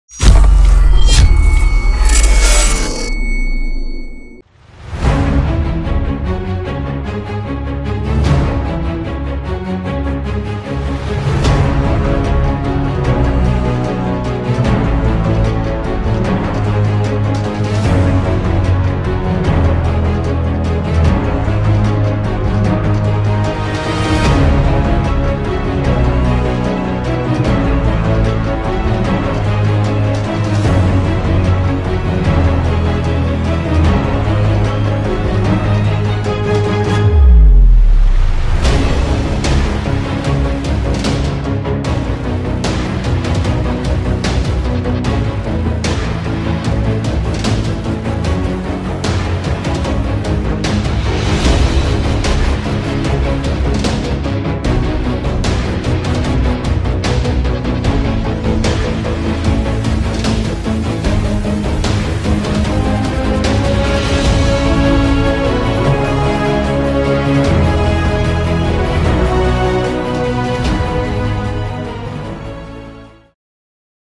Element 3D ||LANDSCAPE||Breakdown || After Effects || LCC VFX
HELLO !
In this work I am just try to create a landscape epic scene.
I used jet strike  packs and red giant particular and try to composite well ,
I also used optical flare